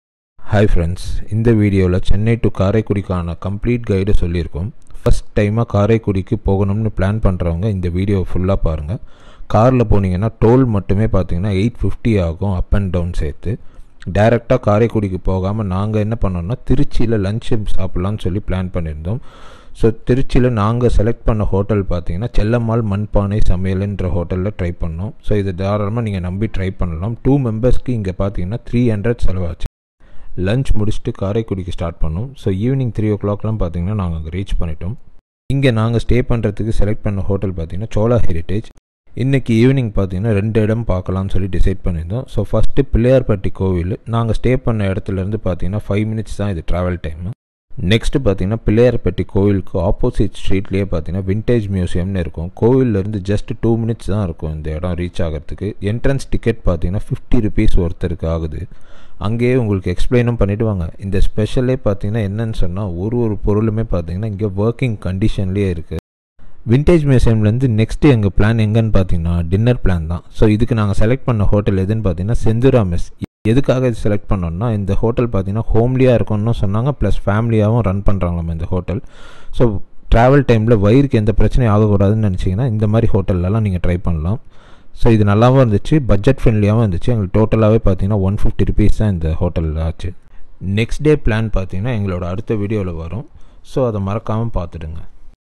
Planned Karaikudi ? | Guide to Karaikudi | Chettinad palace
A complete guide to karaikudi. There will be totally three videos. What to see, where to stay and where to eat. We planned a 3 days trip, toally 16 tolls (Rs.850) Booked rooms through goibibo and makemytrip.
Chennai to karaikudi total 419KMS, it was a 8hrs journey(Including breakfast & lunch breaks) To reach karaikudi we will be going through
so we planned our lunch in Chellammal Manpaanai Samayal
Address: 28, Officers Colony Road, opp. Govt hospital, Puthur, Tennur, Tiruchirappalli, Tamil Nadu 620017
Hours:  Opens 12PM to 3:30PM
for two people it was around 300Rs.
By 3:30PM we reached karaikudi & we booked our room in
CHOLA HERITAGE (Rs.1950 with Breakfast)
Address: No.346/1, Iluppakkudi Road 800mtr From Railway station - 1.5km From Alagappa College, Karaikudi, Tamil Nadu 630003
After a small break we went to two places pillayarpatti temple and CHETTINAD VINTAGE CAR AND CAMERA MUSEUM
Address: 4MG9+7GV, Pillayarpatti, Tamil Nadu 630212
Hours:  Opens 10AM - 5:00PM
Entrance ticket Rs50 per person,All the things are in working condition. Do visit that museum
After that we went to have dinner at Sri sendhura homely mess
Address: 6th St, Subramaniapuram, Vairavapuram, Karaikudi, Tamil Nadu 630002
Hours: 6:00PM - 10:00PM
For two person it was Rs.150/-
Do watch our next video will be covering things to see in karaikudi(athangudi palace,athangudi tiles, chettinad food)